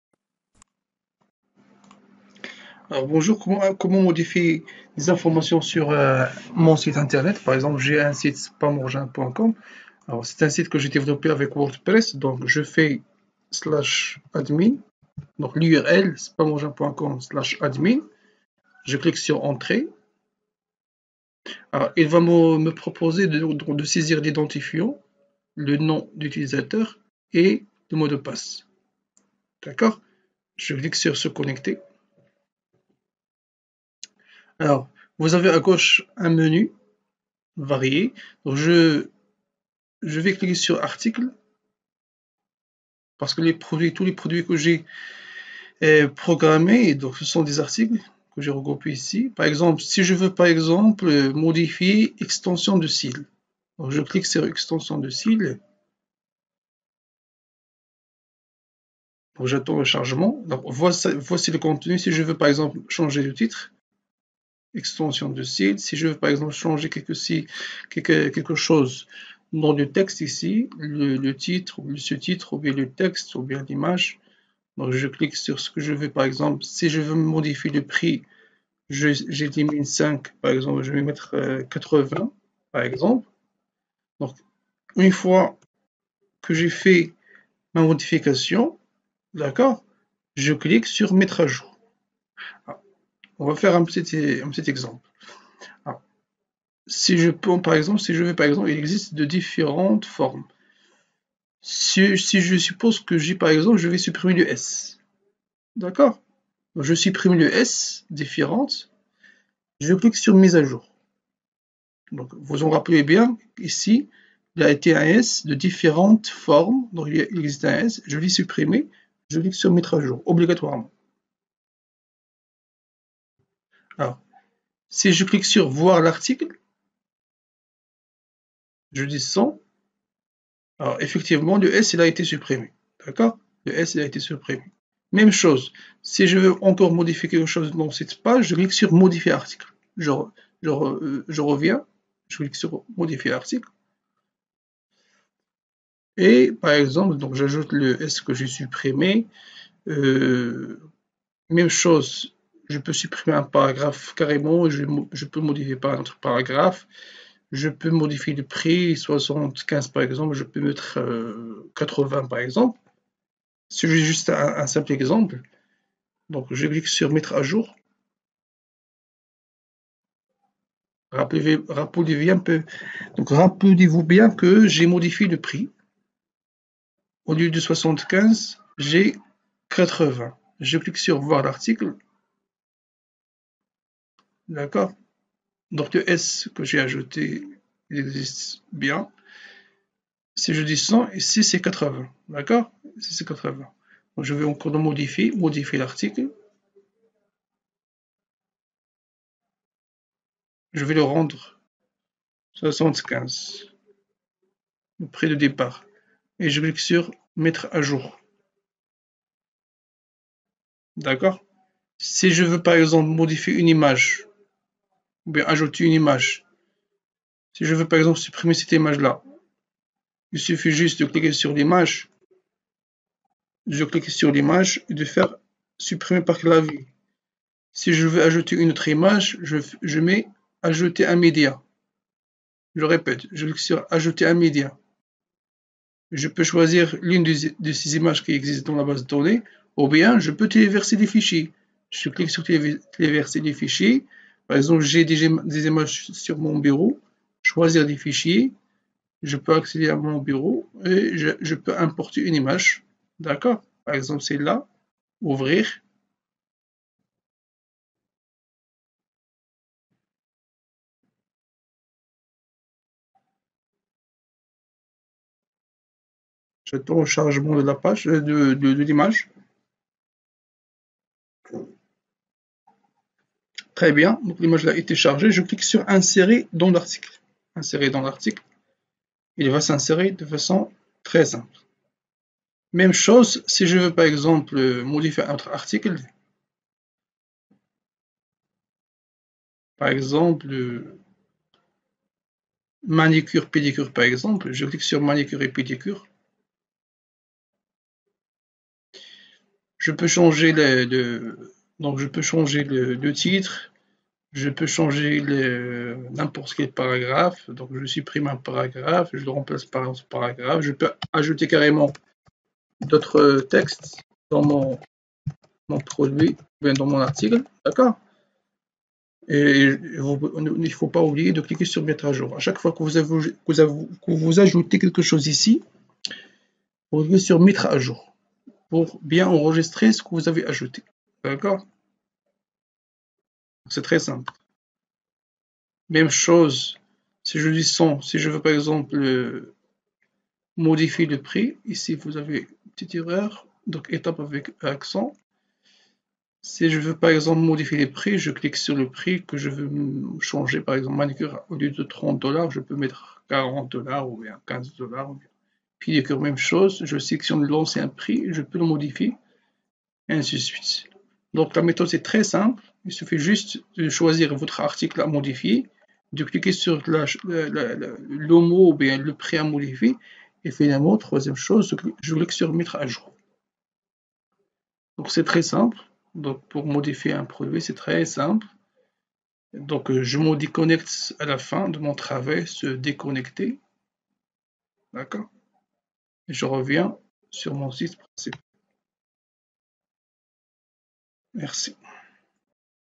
MODIFIER ARTICLE WORDPRESS
J'explique dans cette vidéo comment modifier un article sur un site développé par Wordpress.